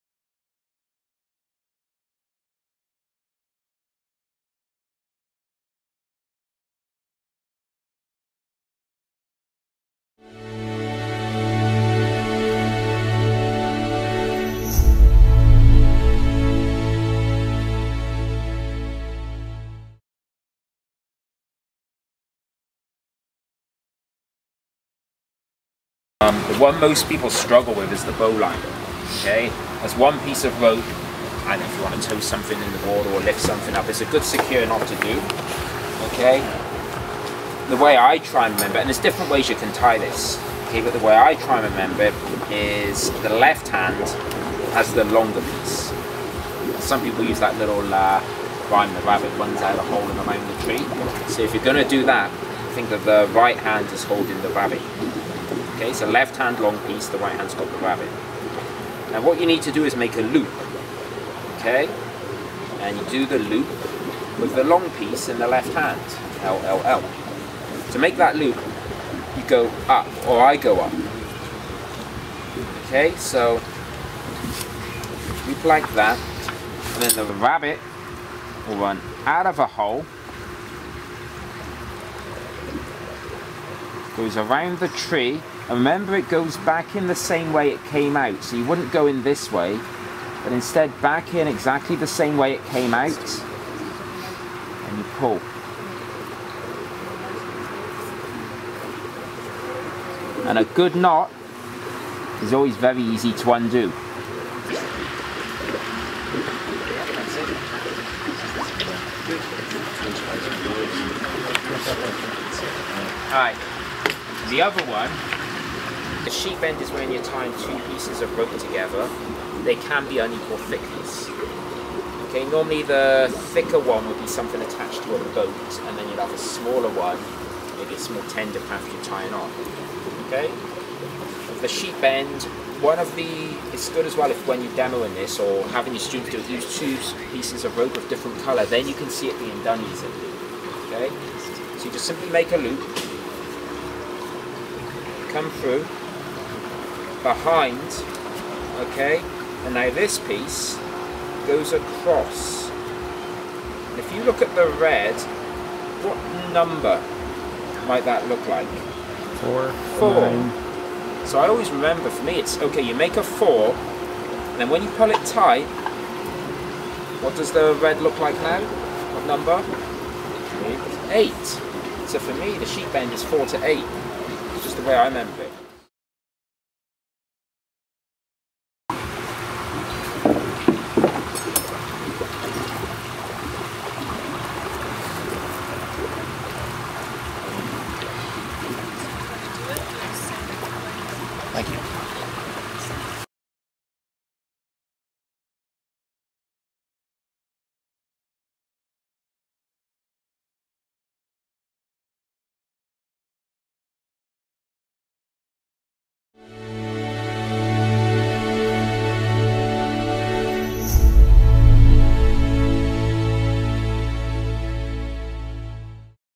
Knot Tying Workshop in PADI IDC
Tying the bowline, 2 half hitches and sheet bend knots in the Utila Dive Centre PADI Scuba Instructor course.